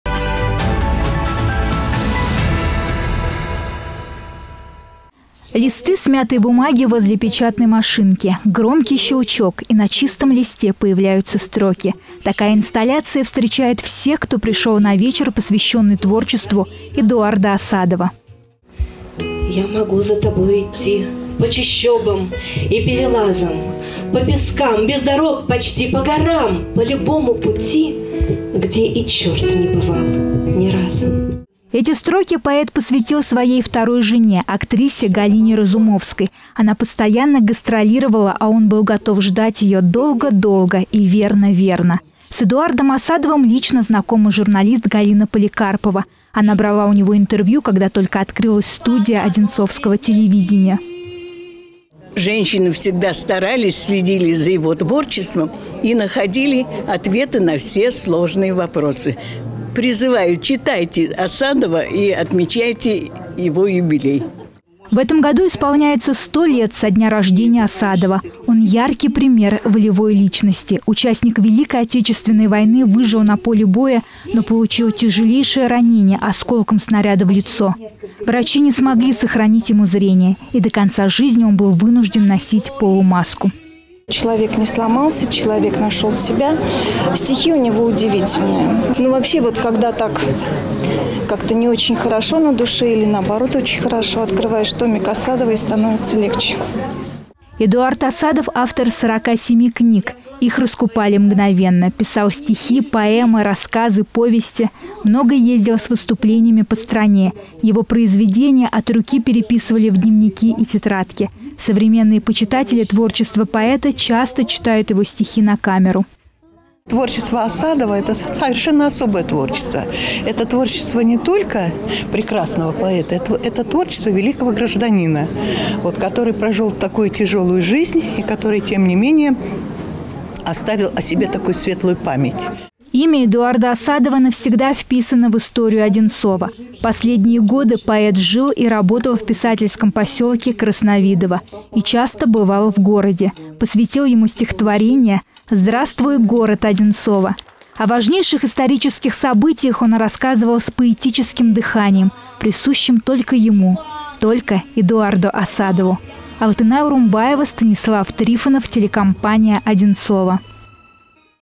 Музыкальная гостиная к 100-летию Эдуарда Асадова
Он завещал захоронить свое сердце на Сапун-горе в Севастополе. В боях за этот город в годы Великой Отечественный войны Эдуард Асадов получил тяжелое ранение. Но, несмотря ни на что, не сломился и навсегда вписал свое имя в историю России. К 100-летию со дня рождения Эдуарда Асадова в Одинцовской библиотеке № 1 прошел вечер, посвященный творчеству советского поэта.